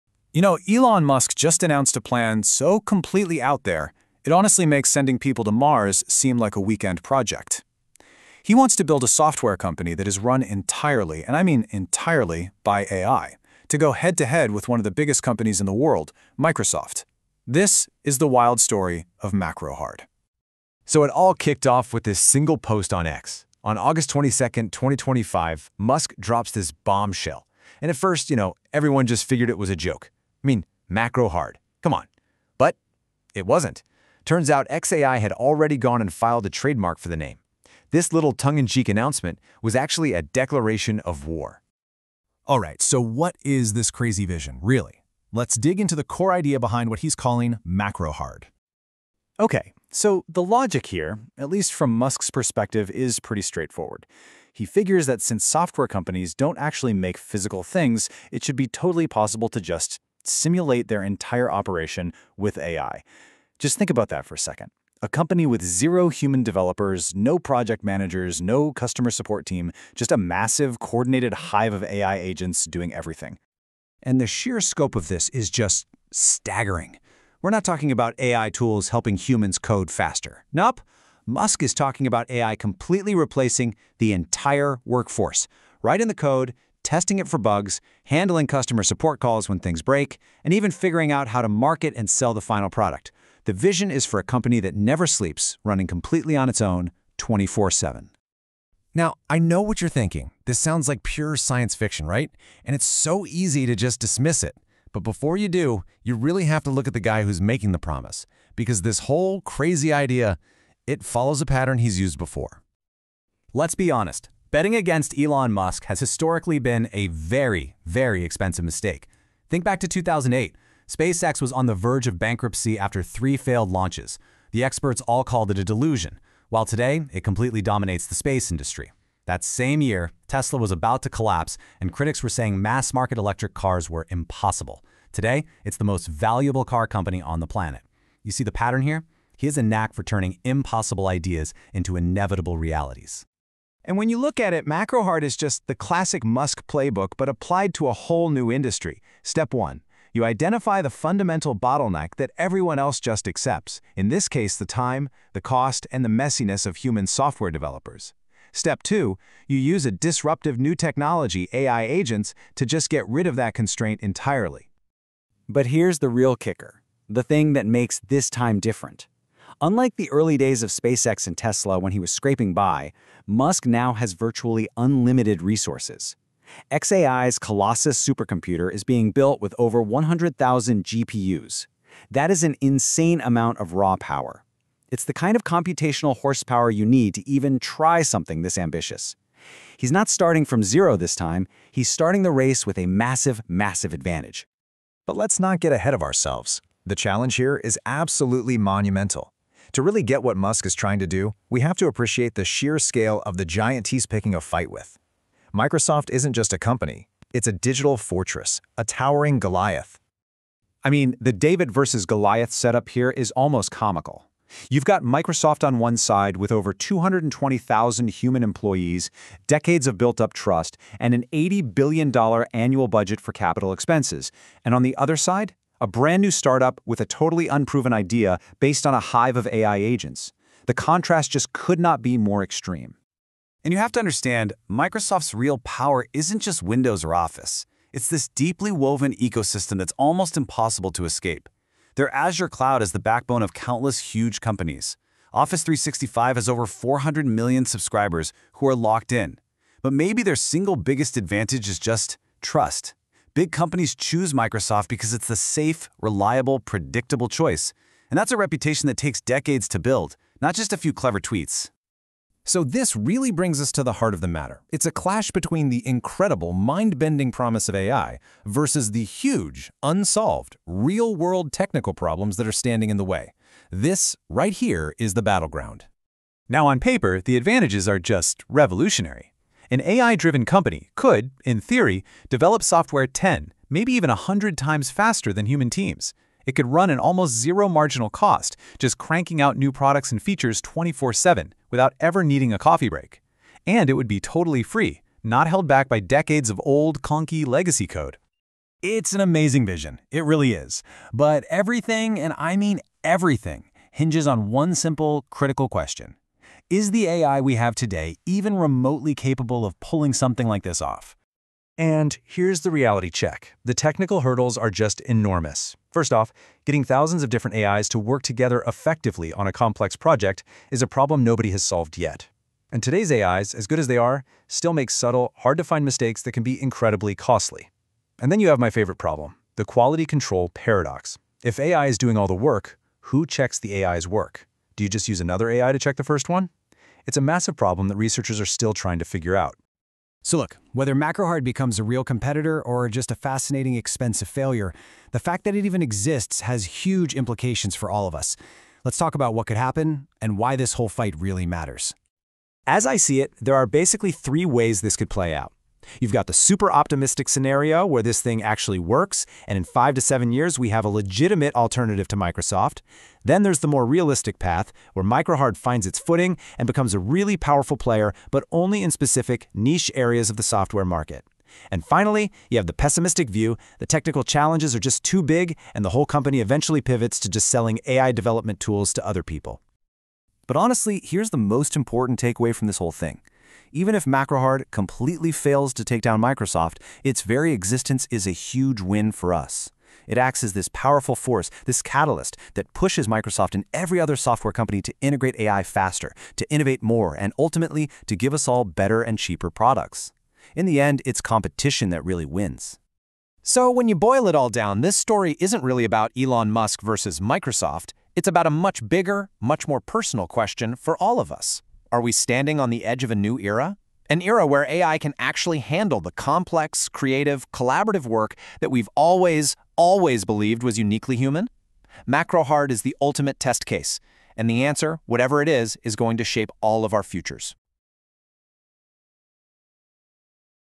Elon Musk vs Microsoft – The Macrohard AI War Begins!
The Elon Musk's ambitious new venture, **Macrohard**, an AI-driven software company designed to compete directly with Microsoft. This enterprise aims to develop software entirely through **AI agents**, eliminating the need for human developers and managers, and handling everything from concept to customer support. The article explores the **potential impact** Macrohard could have on the software industry, drawing parallels to Musk's previous successes with SpaceX and Tesla, which overcame significant challenges to disrupt established markets. However, it also acknowledges the **immense technical and logistical hurdles** that Macrohard must overcome, including the need for advanced AI coordination, unwavering reliability, and overcoming Microsoft's deeply entrenched ecosystem and vast resources. Ultimately, the source concludes that while Macrohard may not destroy Microsoft, it could **force significant innovation** in the industry and prove the viability of AI in complex software development.